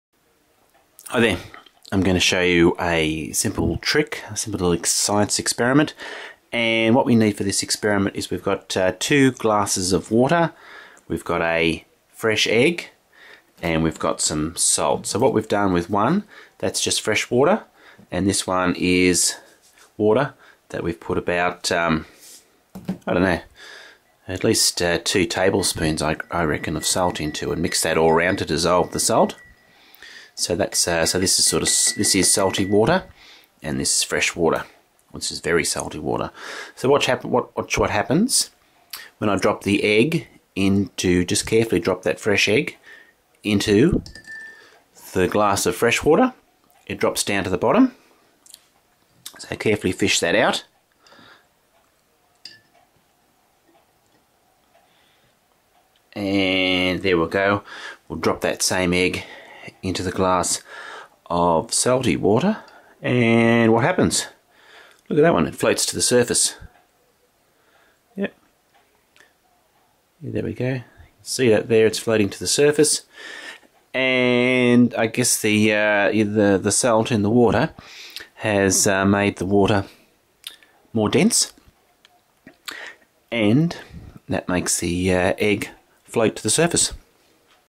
Do Eggs Float in Salty Water? Do Eggs Sink in Fresh Water? Simple Science Experiment
Salty water is more dense than salt water, so fresh eggs will sink in fresh water and float in salty water. However the salty water shown in this video is much more salty than regular sea water.  I used around 2 tablespoons of salt in a cup of water to get the egg to float.